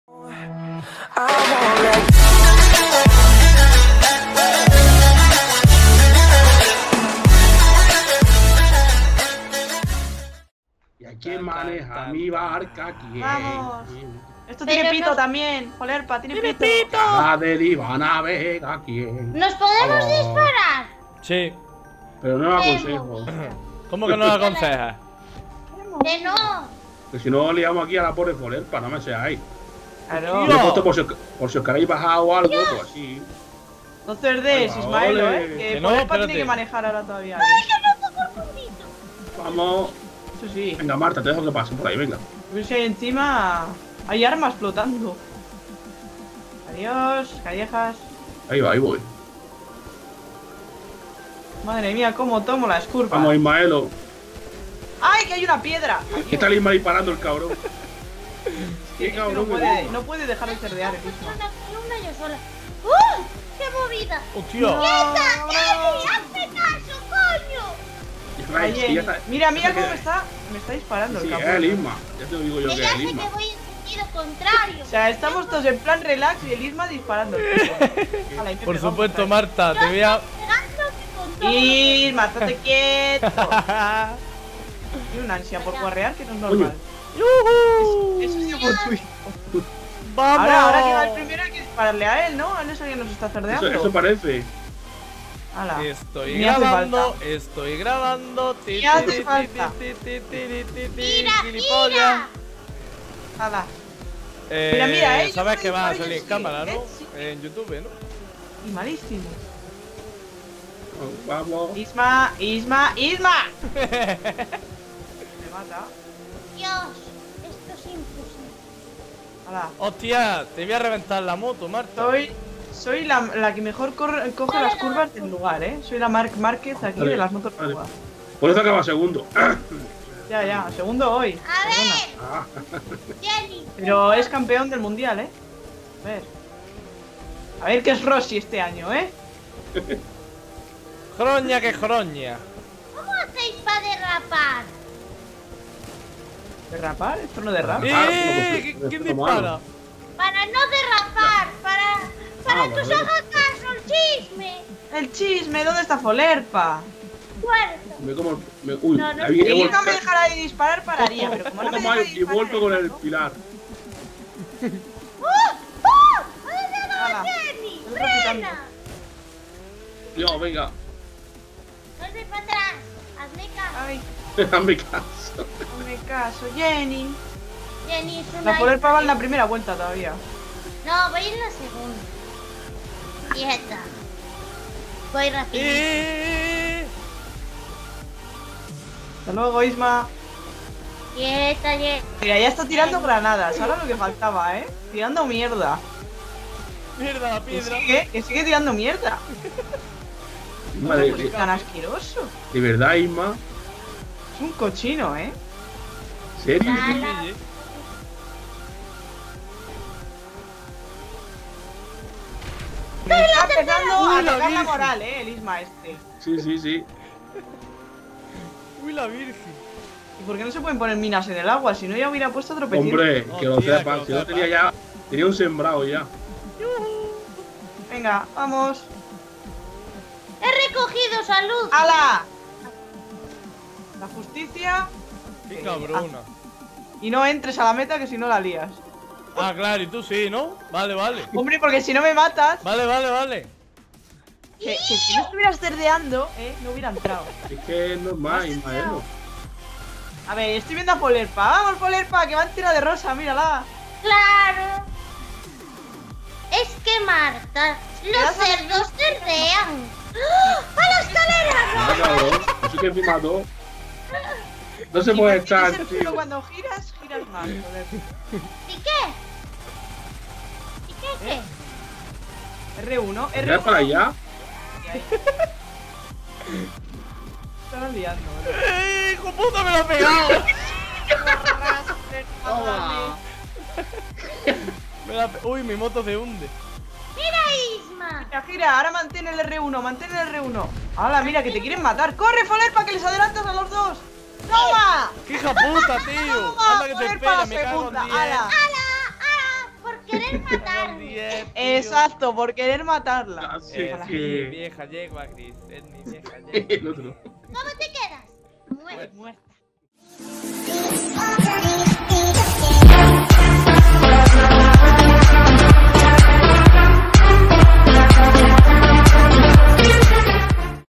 INTENTO GUARREAR PERO NO PUEDO... GRUPO FOLERTUKALLEJAS!
SEGUIMOSSSSSS CON LA CARRERAS ESTA VEZ MOTOS DE AGUA! NO CONSIGO GUARREAR... CON LO QUE A MI ME GUSTA PERO EN FIN! ESPERO QUE OS GUSTE! UN ABRAZO!!!